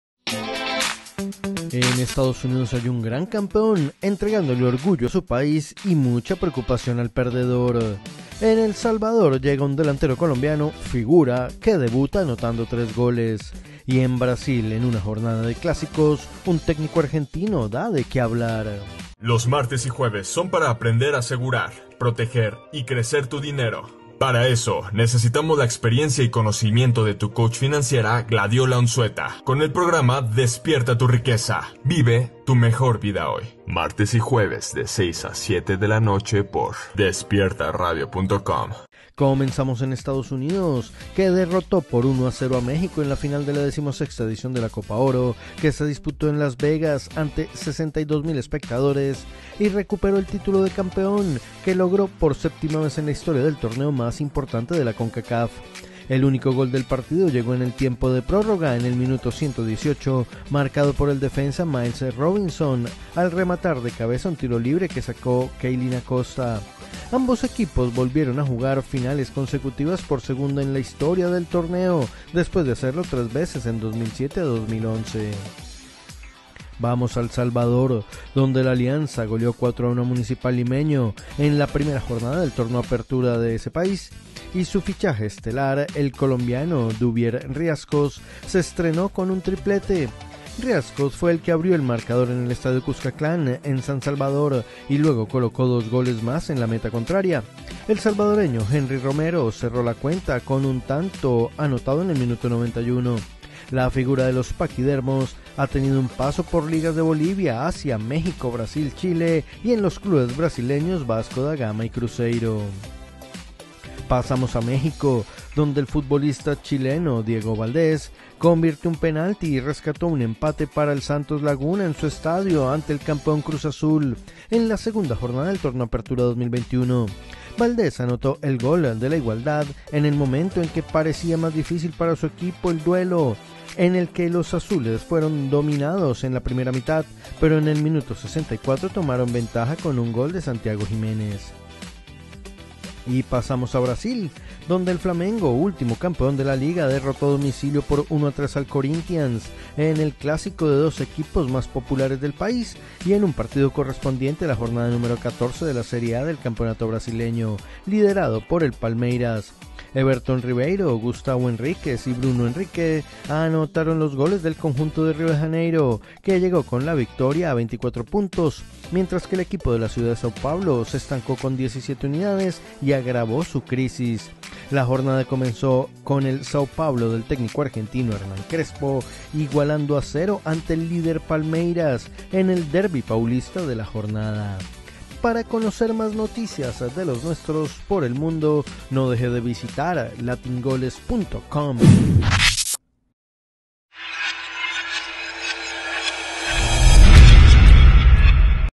En USA hay un campeón lleno de orgullo
En Estados Unidos hay un gran campeón entregándole orgullo a su país y mucha preocupación al perdedor, en El Salvador llega un delantero colombiano, figura, que debuta anotando tres goles y en Brasil en una jornada de clásicos y técnico argentino da de qué hablar.